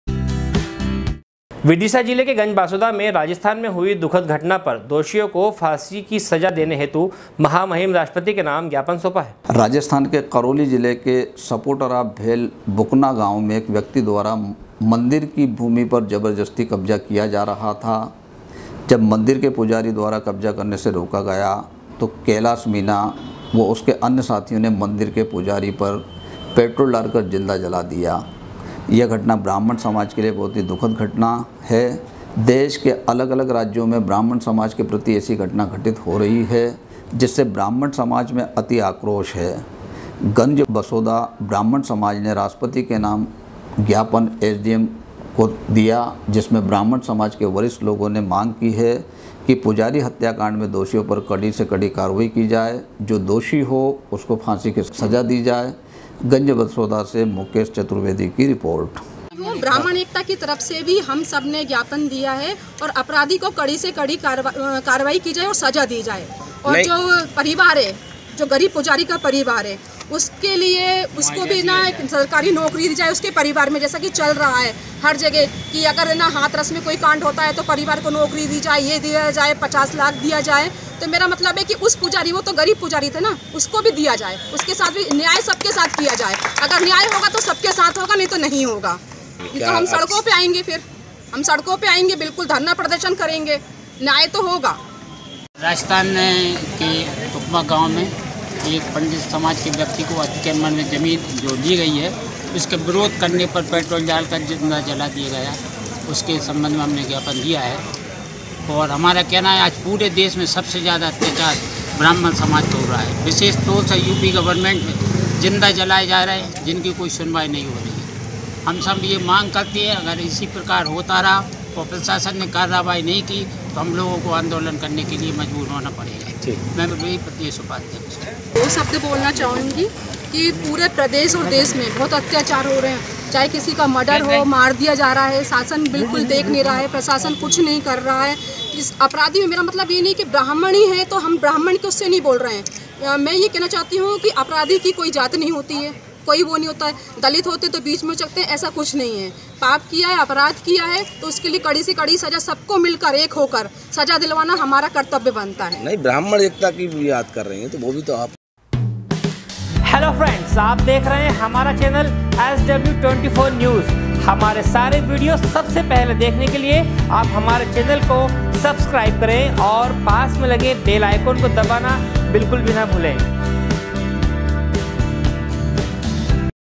SW 24 News Madhyapradesh
विदिशा जिले के गंजबासौदा में राजस्थान में हुई दुःखद घटना पर दोषियों को फाँसी की सजा देने हेतु महामहीम राष्ट्रपति के नाम ज्ञापन सौपा। गंजबासौदा से मुकेश चतुर्वेदी की रिपोर्ट